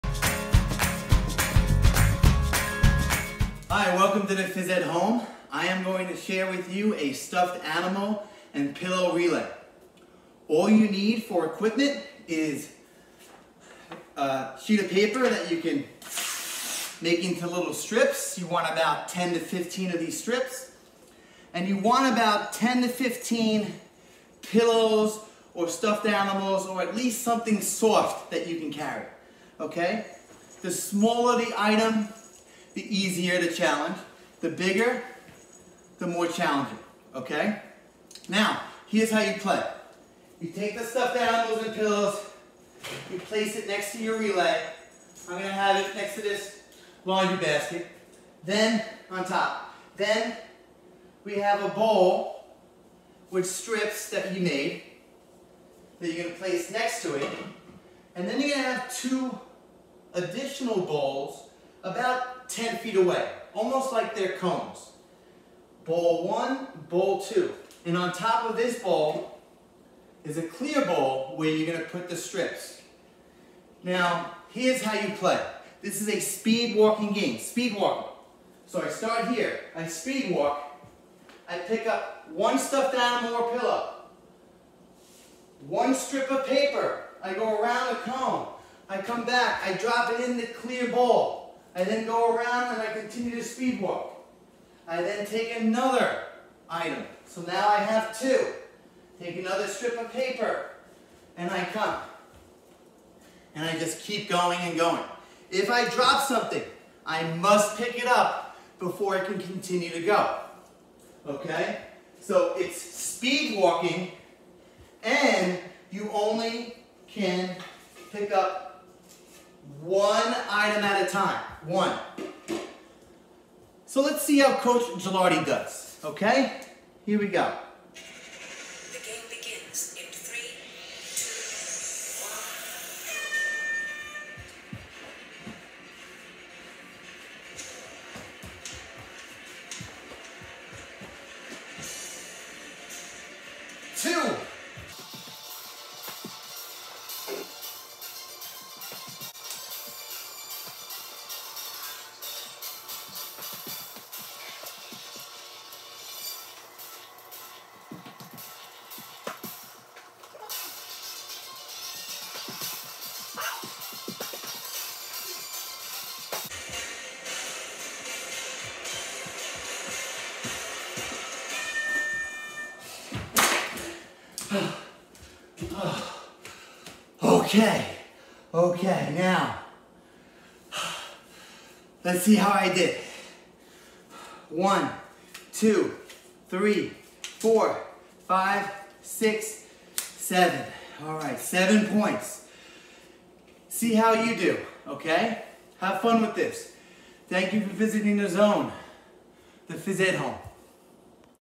PE At Home: “Stuffed Animal/Pillow Relay” Challenge
Here is a fun at-home PE activity that uses locomotor skills while holding your favorite stuffed animals. You have one minute to see how many stuffed animals or pillows you can hold while traveling from one spot to another before the time runs out. Each lap, you need to carry another stuffed animal and a lap strip of paper. It is silly and challenging all at once. Have fun alone or challenge a family members.